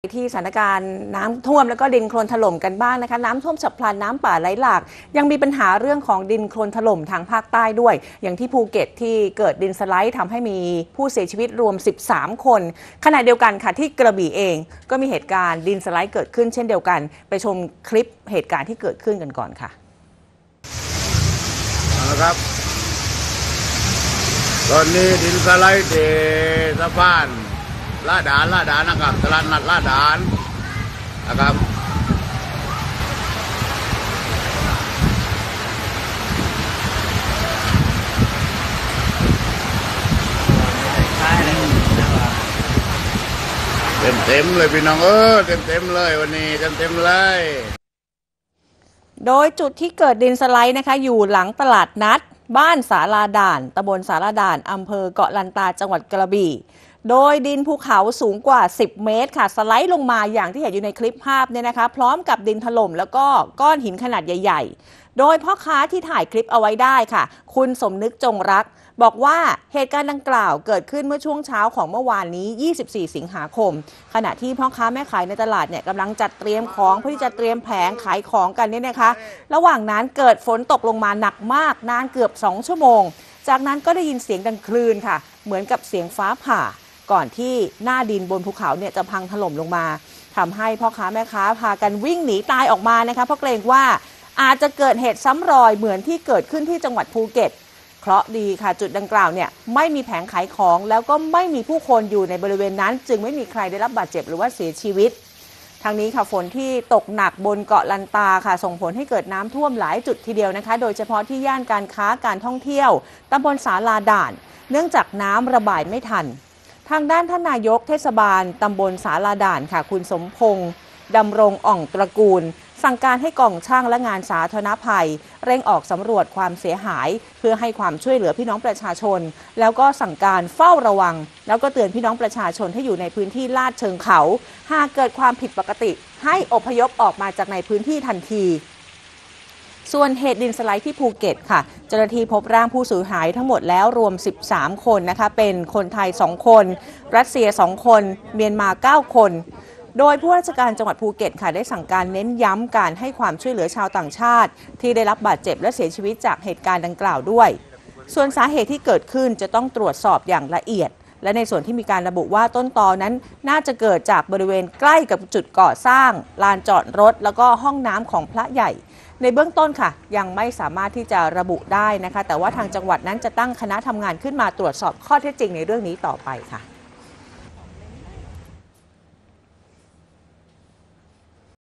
ดินสไลด์ ที่เกาะลันตา จ.กระบี่ ข่าวเที่ยง วันที่ 25 สิงหาคม 2567
สถานการณ์ในพื้นที่ภาคใต้ นอกจากเรื่องของน้ำท่วมฉับพลันและน้ำป่าหลากแล้ว  ยังมีปัญหาเรื่องดินโคลนถล่ม อย่างที่ภูเก็ตที่เกิดดินสไลน์ มีผู้เสียชีวิตรวม 13 คน   ขณะเดียวกันที่กระบี่  ก็มีเหตุดินสไลด์เช่นกัน